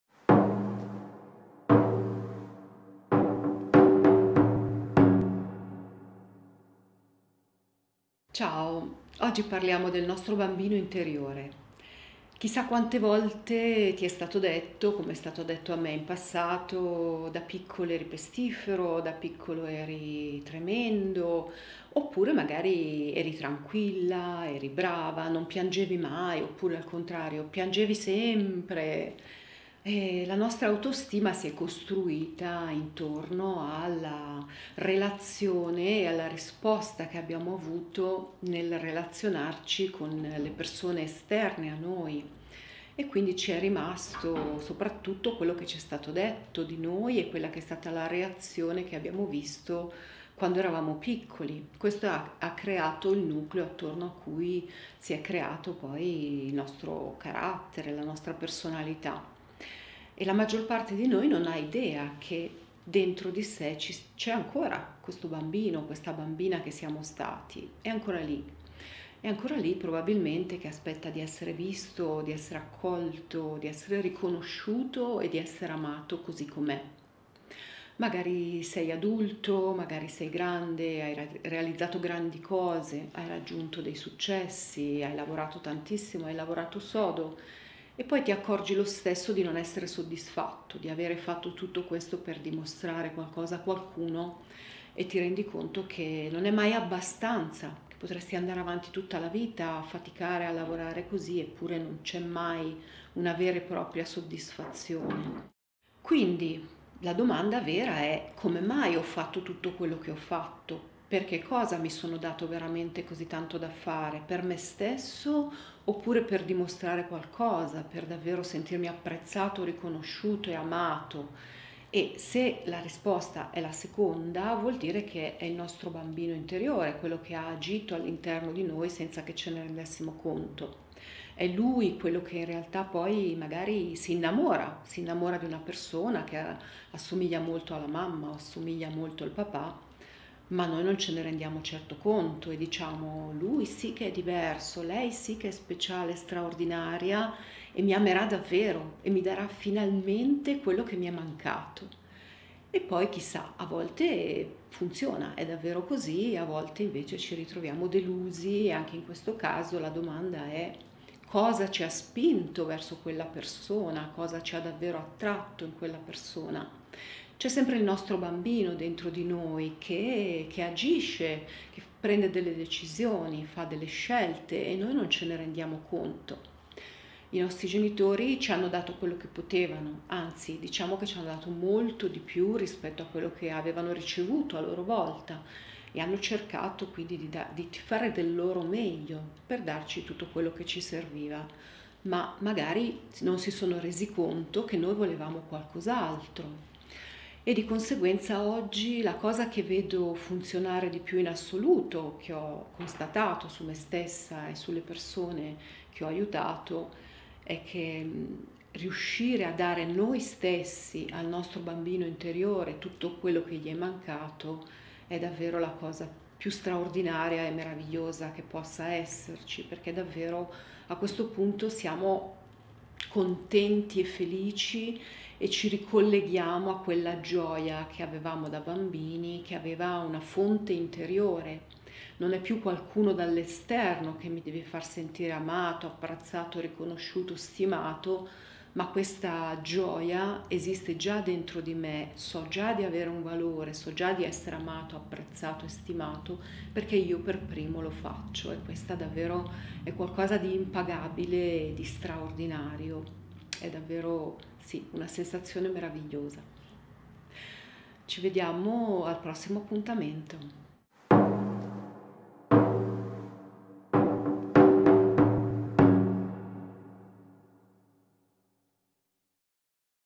Valeria Pisano P05
In questa puntata Valeria Pisano ci parlerà del BAMBINO INTERIORE